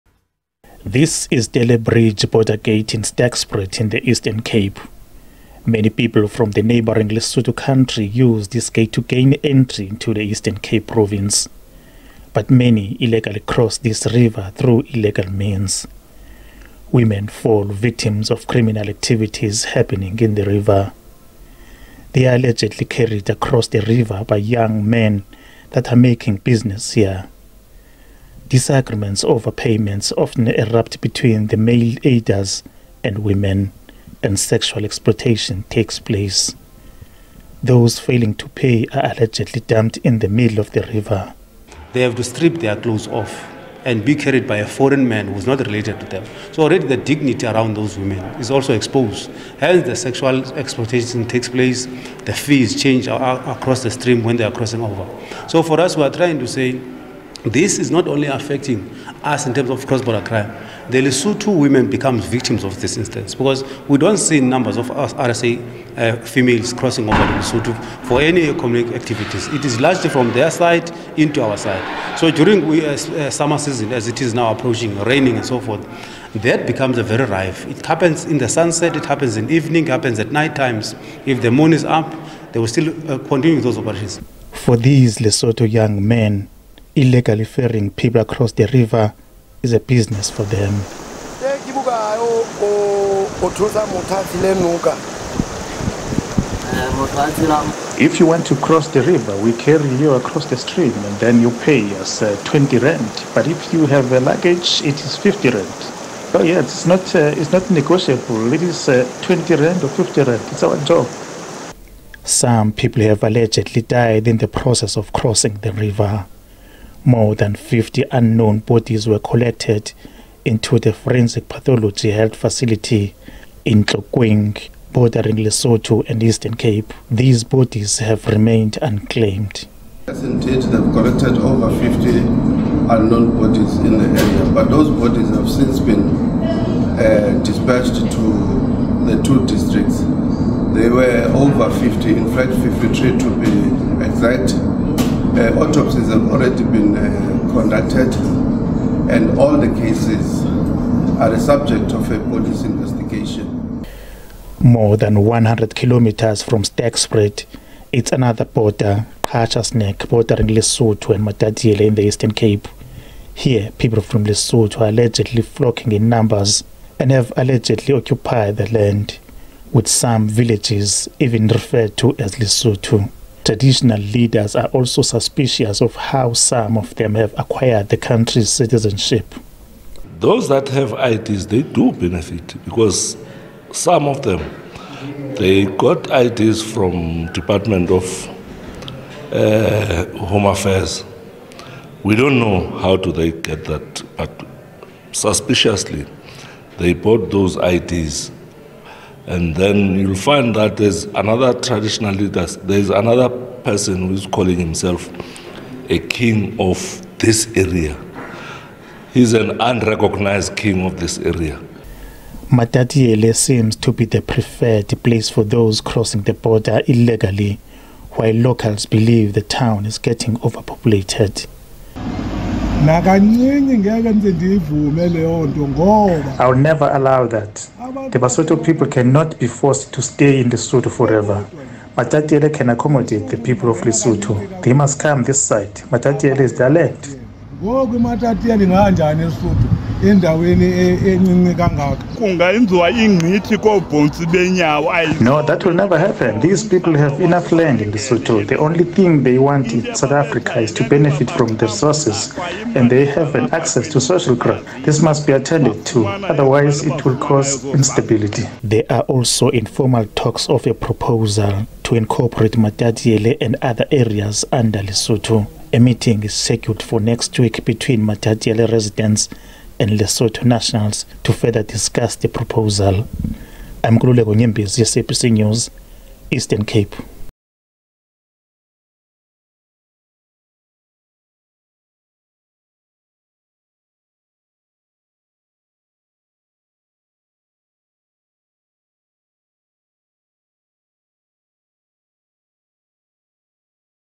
50 bodies retrieved from Telle River following alleged attempts to enter SA illegally
The Telle Bridge Border gate between Sterkspruit in the Eastern Cape and Lesotho has become the latest entry point for illegal crossing for Lesotho nationals using the river into the country.

 The border gate is also a den for crime where women have been allegedly sexually assaulted crossing the river, while bogus traditional leaders are selling land to Lesotho nationals.

Our reporter Nkululeko Nyembezi and video journalist Sinethemba Blandile compiled this report.